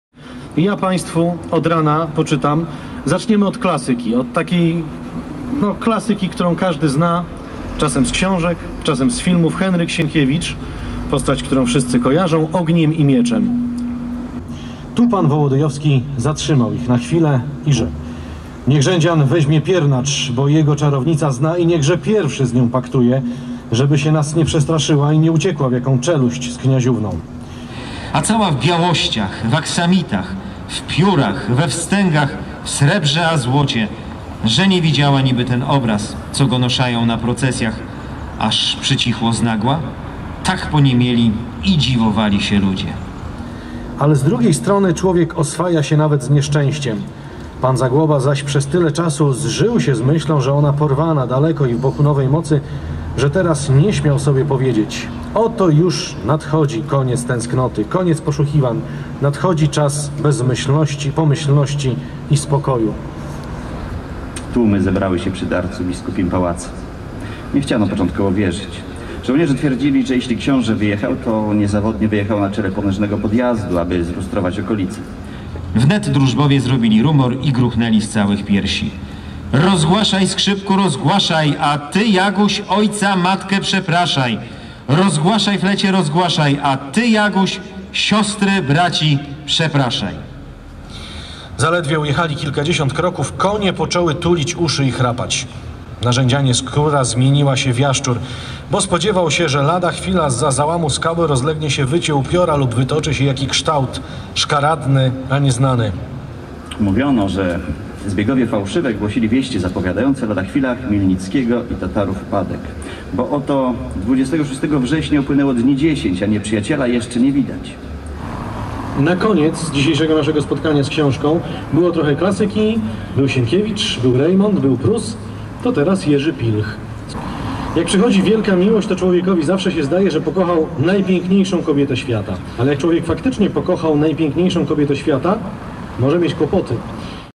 Dziennikarze Radia Olsztyn czytali książki w Nidzicy! 23.04.2014r.
Dziś w Światowym Dniu Książki Radio Olsztyn rozmawia ze słuchaczami o ostatnio przeczytanych książkach.

Nasi dziennikarze od rana byli w Nidzicy. W centrum miasta dziennikarze Radia Olsztyn czytali będą mieszkańcom miasta znane i lubiane książki.Każdy mógł też wypić z nami poranną kawę.

Jak wynika z najnowszych danych w Polsce ciągle spada czytelnictwo. Według badań Biblioteki Narodowej, 60 procent Polaków przez rok nie przeczytało ani jednej książki.